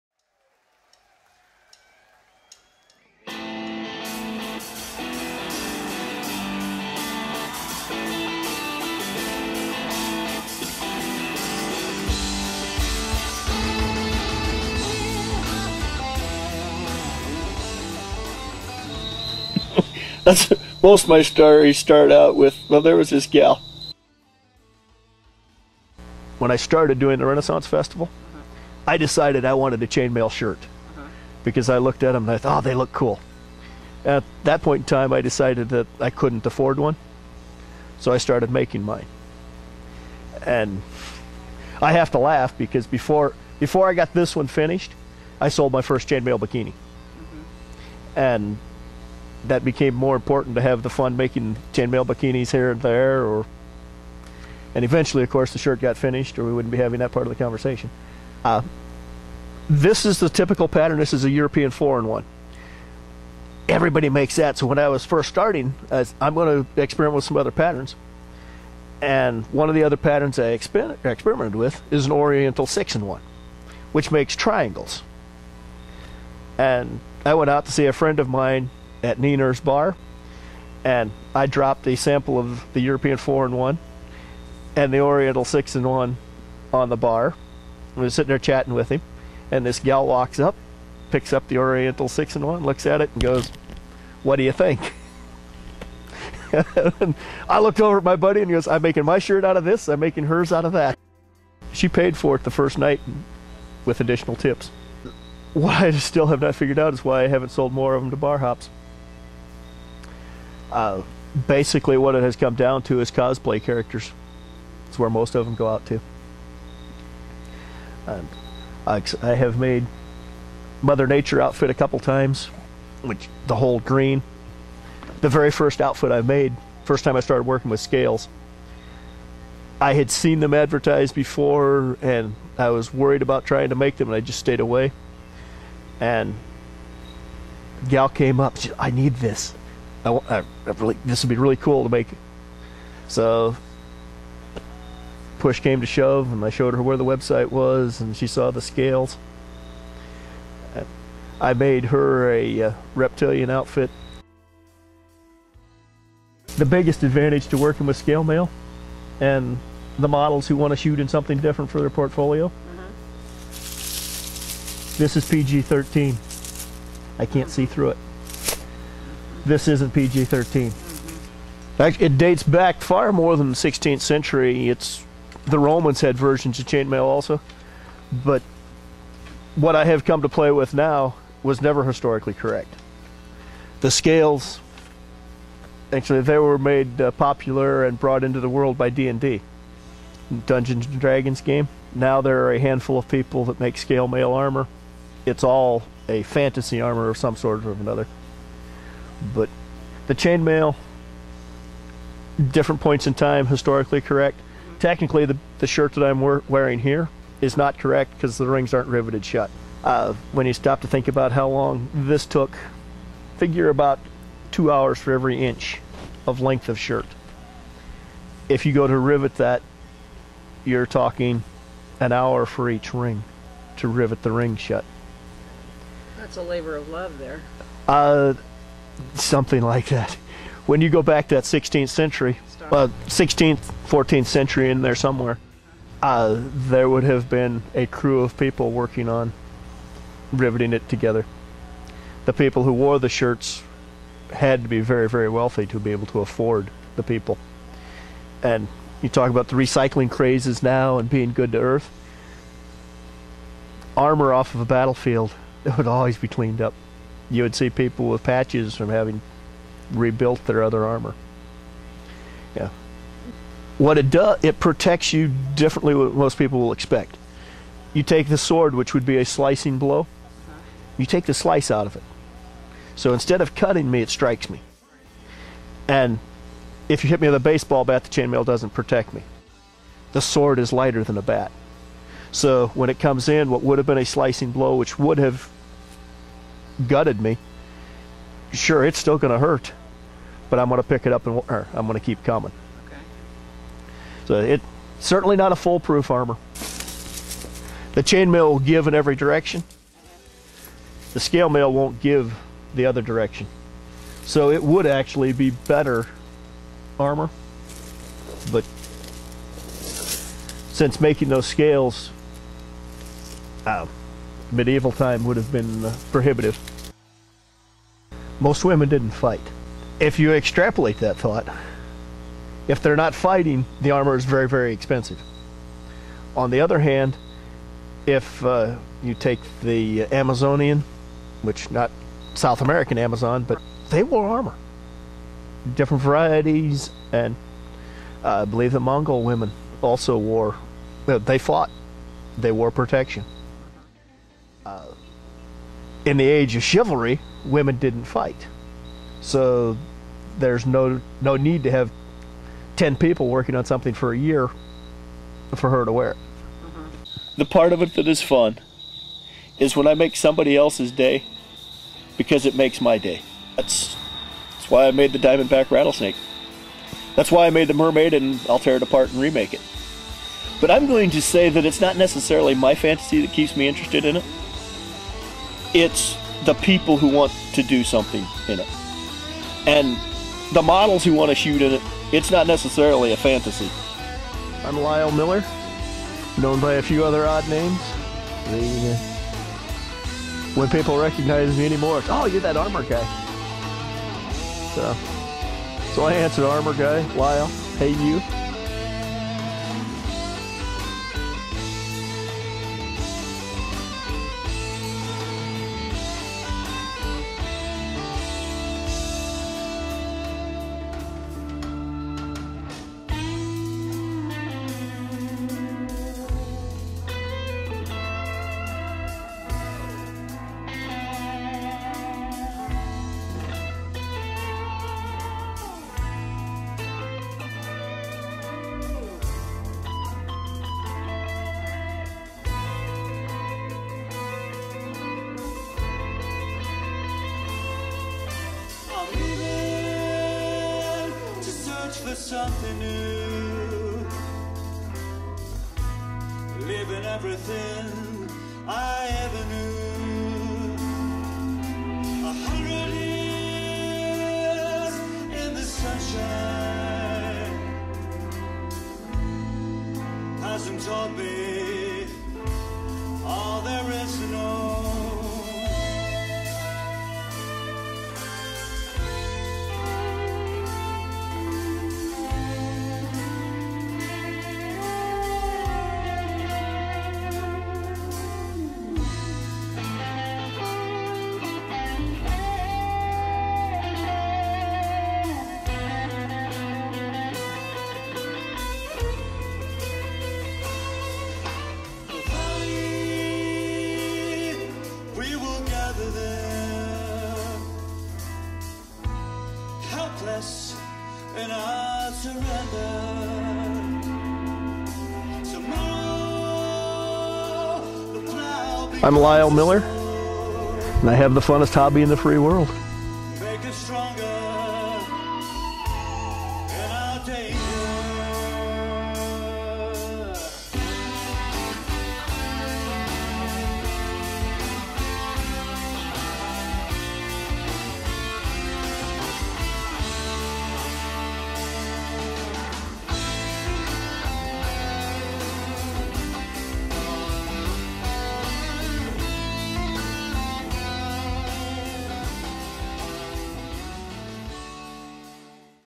Women in Chains III
Lyle Miller discusses his passion for chain maille and fanasty armor.